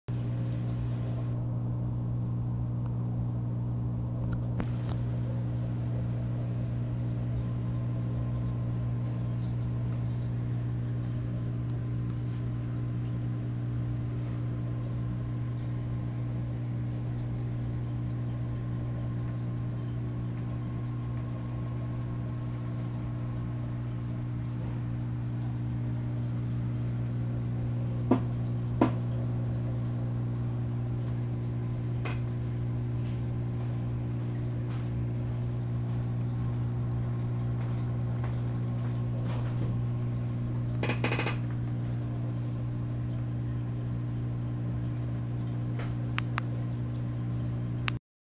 Platinum clownfish
My 30 gallon aquarium with Platinum clownfish Pair